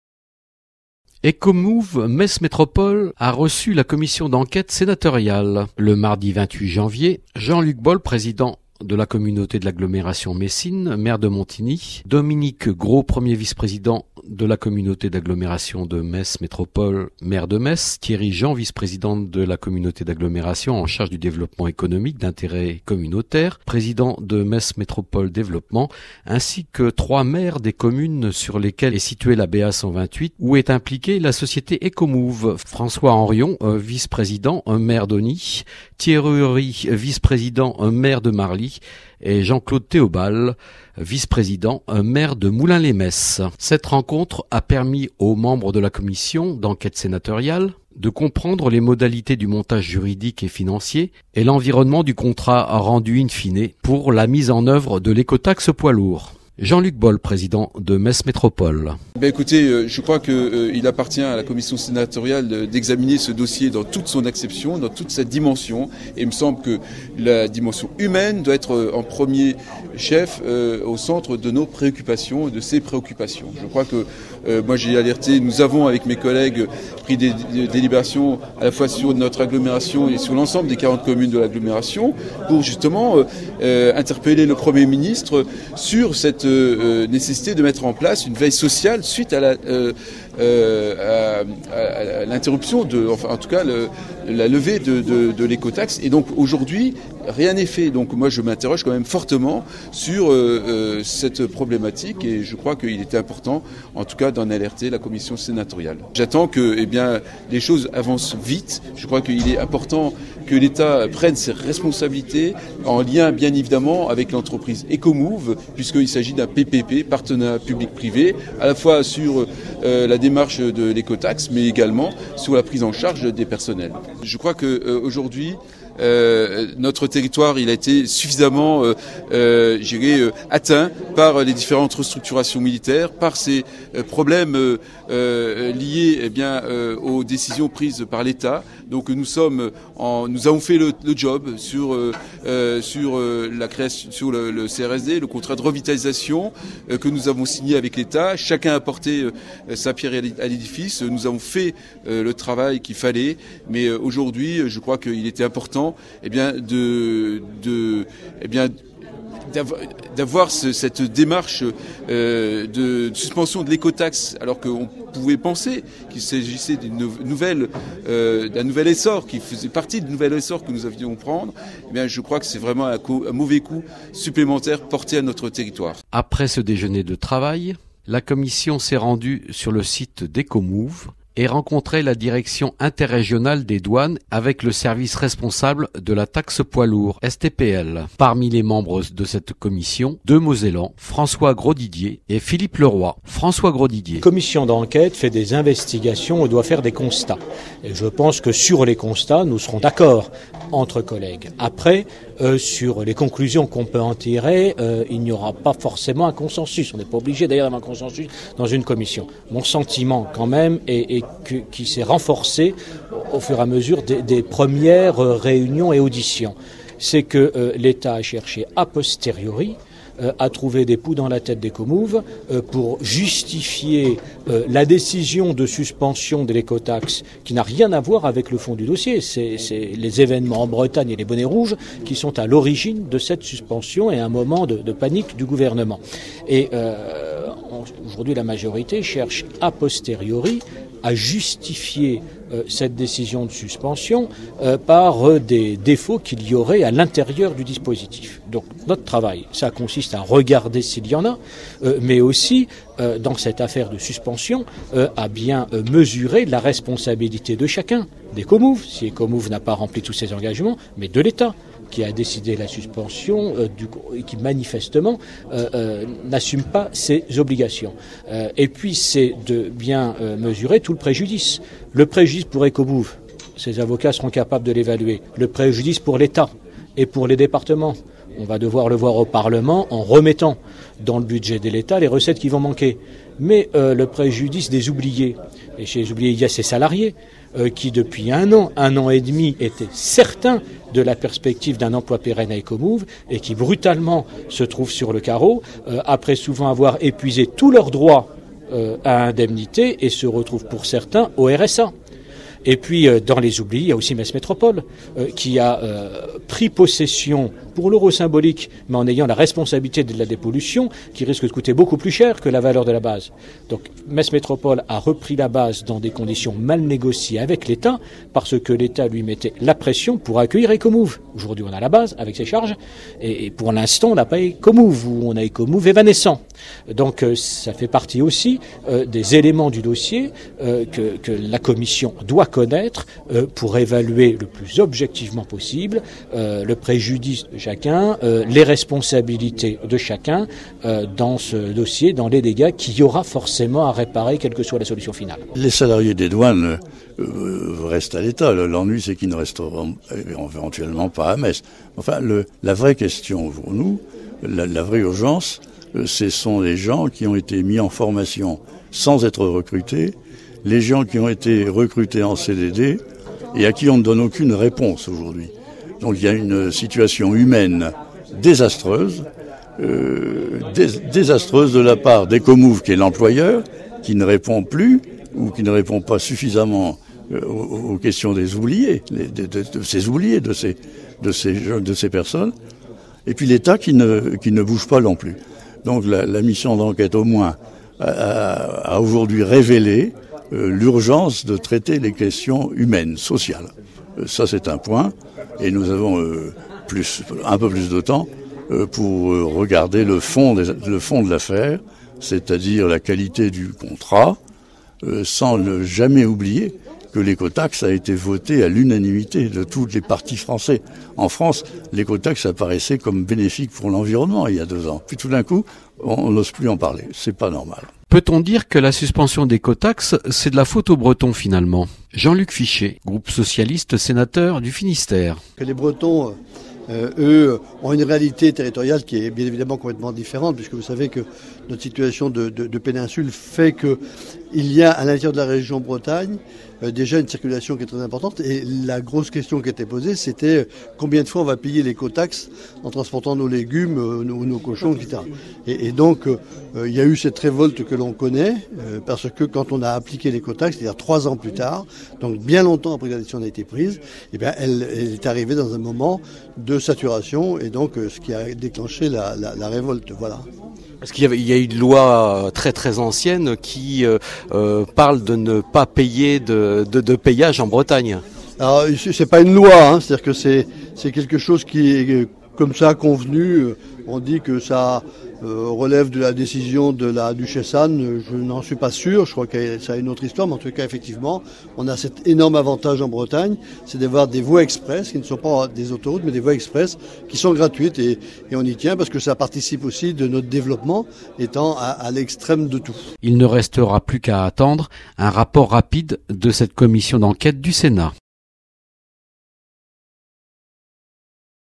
ECOMOUV la commission d'enquête Sénatoriale à Metz
Ce mardi 28 janvier au Siège de l'Agglomération, Jean-Luc BOHL, Président de Metz Métropole - Maire de Montigny-lès-Metz, à reçu, les membres de la commission d'enquête Sénatoriale « sur les modalités du montage juridique et financier et l'environnement du contrat retenu in fine pour la mise en oeuvre de l'écotaxe poids lourds ».
Cette commission sera présente sur le territoire jusqu'au soir de manière à visiter le site Ecomouv' et à rencontrer la Direction interrégionale des douanes avec le Service responsable de la Taxe Poids Lourds (STPL).
Cette rencontre a permi permettra d'évoquer l'implantation sur le site de l'ancienne BA de la société Ecomouv' dans le cadre des compensations militaires et la très grande inquiétude des Maires de Metz Métropole face à la suspension de l'Ecotaxe et ses conséquences en termes d'emploi et d'activité sur le territoire.
Membres de la commission d'enquête Sénatoriale
Mme Marie-Hélène Des ESGAULX, présidente -- groupe UMP -- Sénateur de la Gironde
Mme Virginie KLÈS, rapporteur -- groupe Socialiste -- Sénatrice d'Ille-et-Vilaine
M. Jean-Luc FICHET -- groupe Socialiste -- Sénateur du Finistère
M. Jean-Jacques FILLEUL -- groupe Socialiste -- Sénateur d'Indre-et-Loire
M. François GROSDIDIER -- groupe UMP -- Sénateur de la Moselle
M. Yves KRATTINGER -- Groupe Socialiste -- Sénateur de la Haute-Saône
M. Philippe LEROY -- groupe UMP -- Sénateur de la Moselle
M. Michel TESTON -- groupe Socialiste -- Sénateur de l'Ardèche